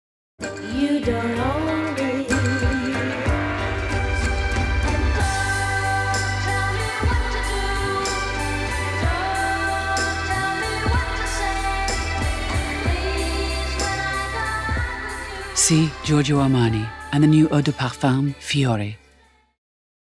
Giorgio Armani - Sì fiori Eau de Parfum
Sì ist die Duft gewordene Hommage von Giorgio Armani an die moderne Weiblichkeit – eine Ode an die Freiheit, die Liebe und das Leben.

2013 feierte Sì Eau de Parfum Premiere.
2018 folgte das leidenschaftlich vibrierende Sì Passione.
2019 eröffnet die Sì Saga jetzt ein neues, florales Kapitel mit Sì Fiori Eau de Parfum.